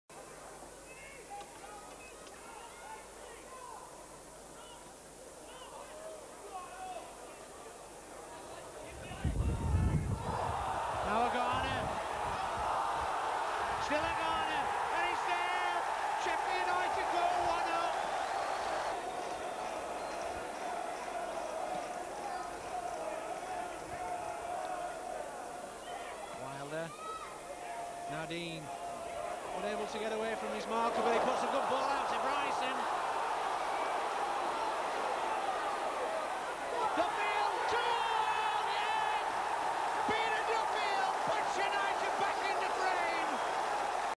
Sheffield United v Brentford, April 22nd 1989
Sheffield United v Brentford, April 22nd 1989. After Tony Agana puts United in front, the Blades need a late goal by substitute Peter Duffield to rescue a point.